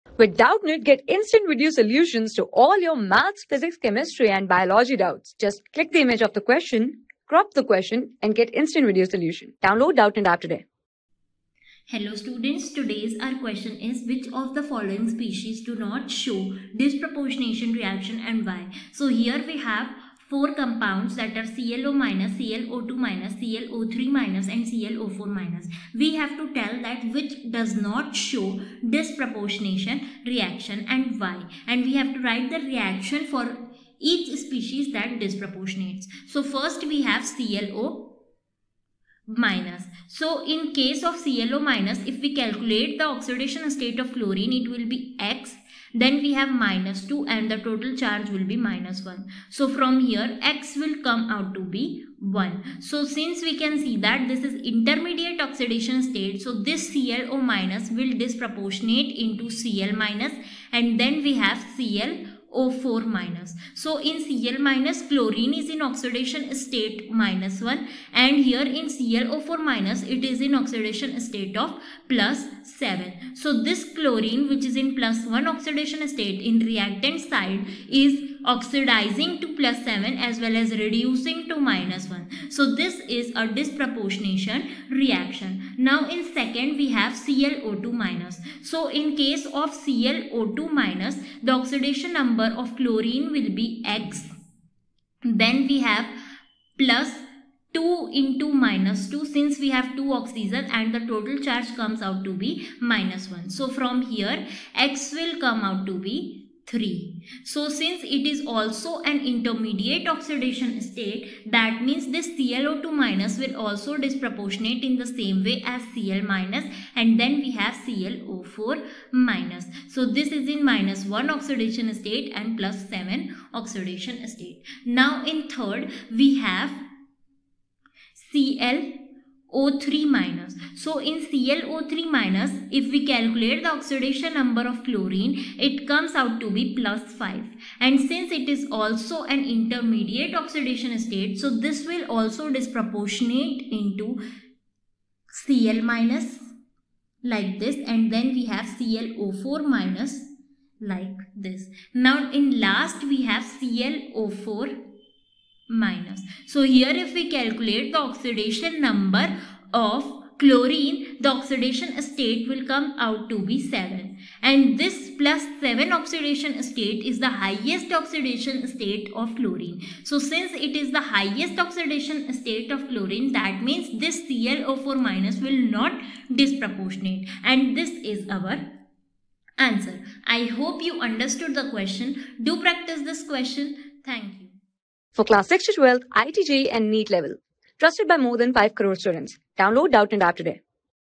Which of the following species, do not show disproportionation reaction and why ? ClO^(-) , ClO_...
Which of the following species, do not show disproportionation reaction and why ? ClO^(-) , ClO_(2)^(-) , ClO_(3)^(-)  and ClO_(4)^(-) Also write reaction for each of the species that disproportionates.
Class: 12
Subject: CHEMISTRY
Chapter: REDOX REACTIONS
Board:IIT JEE

👉 Have Any Query? Ask Us.